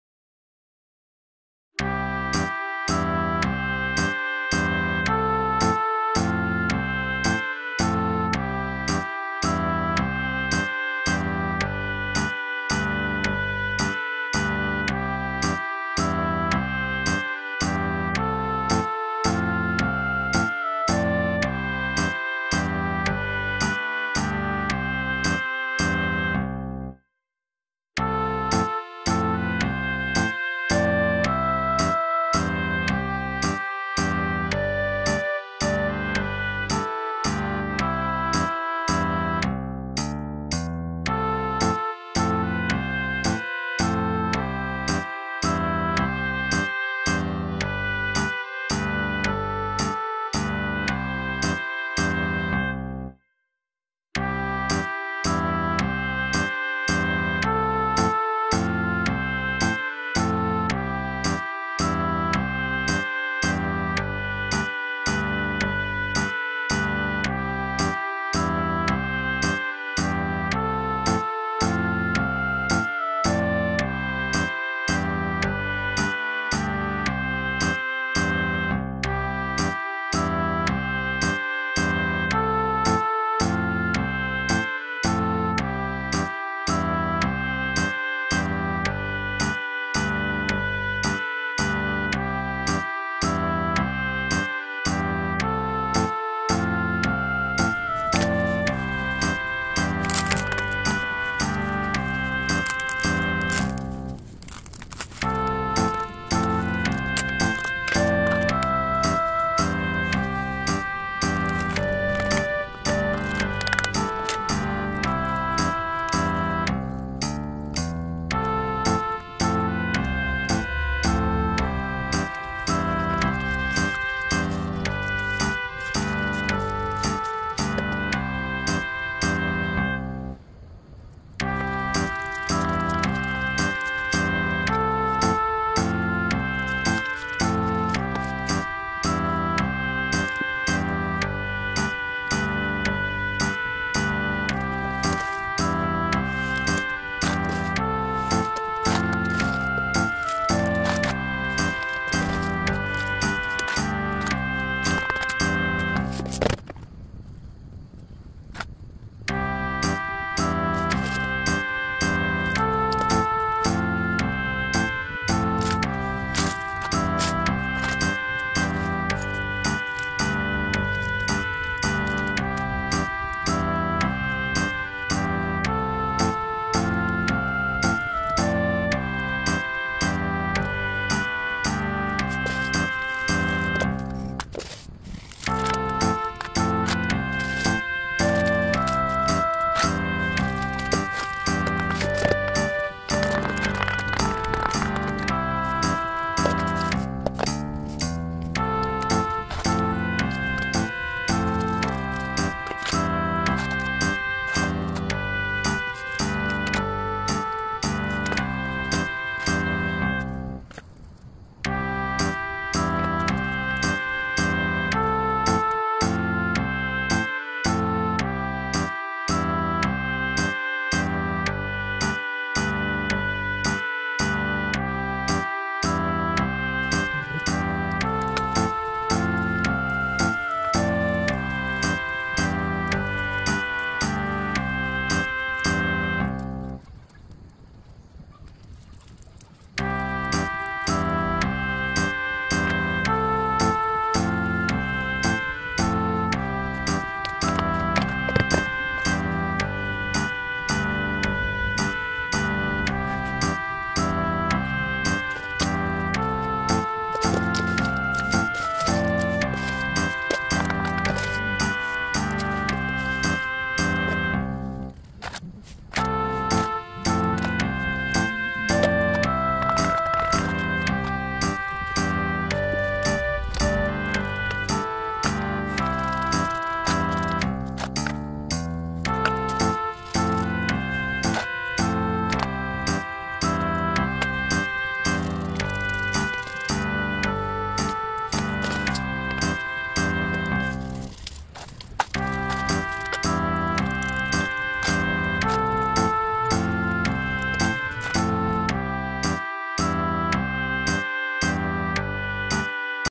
1002 오늘도분갈이 깍지없는 다육생활
깍지없는 다육생활이란 결코 쉬운일이 아닙니다.저의 케어가 깍지가 올틈이 없는것 같아요^^